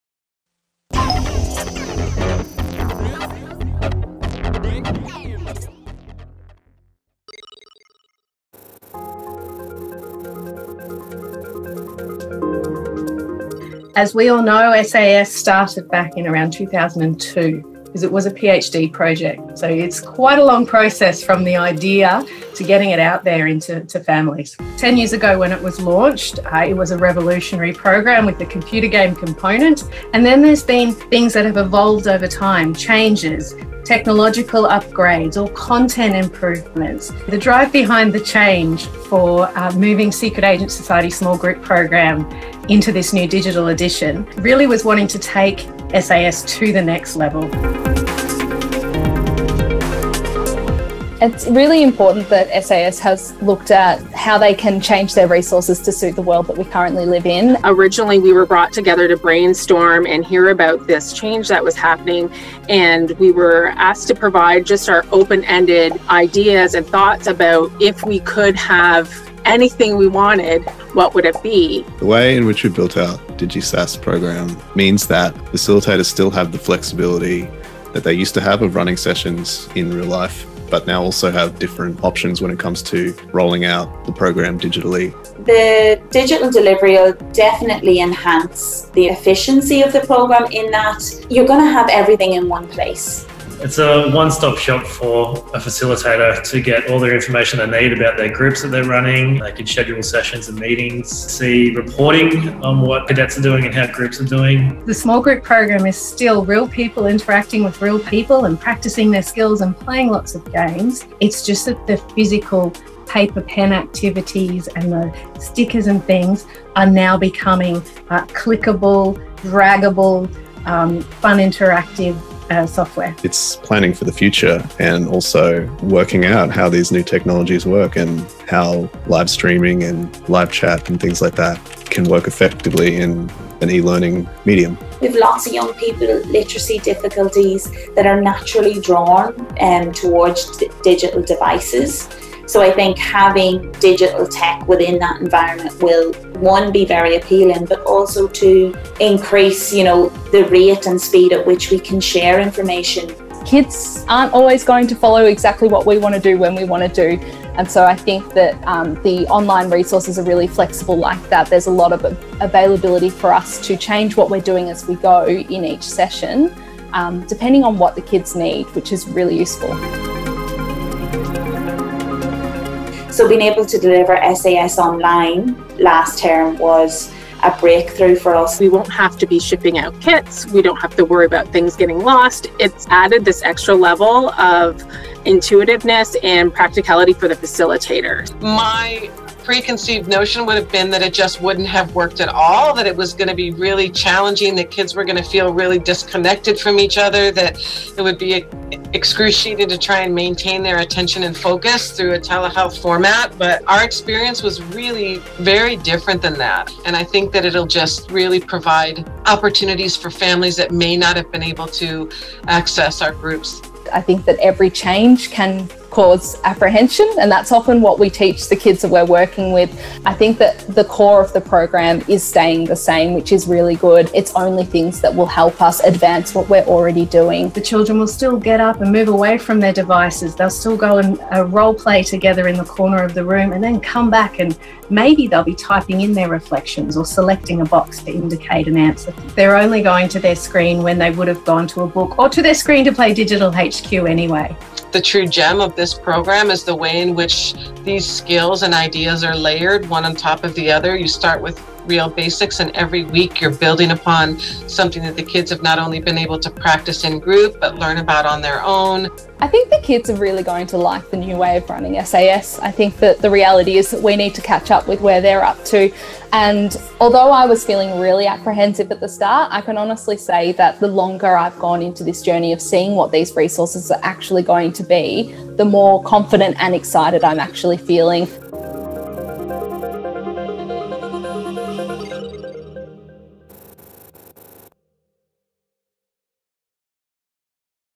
Introducing the New SAS Small Group Program
For the past 10+ years the SAS Small Group Program and you, our Facilitators, have been transforming lives across the globe by empowering children to feel happier, calmer and braver. Now it is time to step into the future.